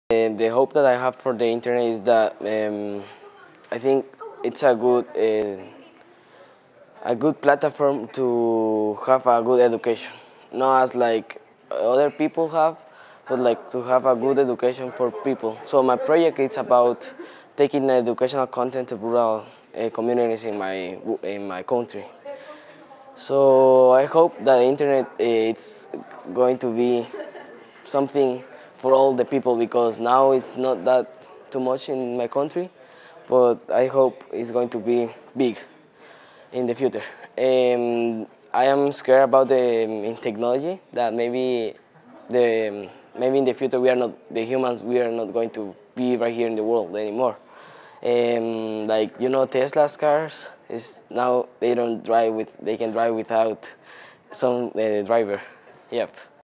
ISOC 25th 25 Under 25 Honoree: Diego Cordova
Diego Cordova was recognized as a 25 Under 25 awardee by the Internet Society for working to enable access to educational materials online. He says the Internet is an important platform for education. Recorded by Elon University's Imagining the Internet Center at the Internet Society's Internet Hall of Fame Induction 2017 in Los Angeles, California.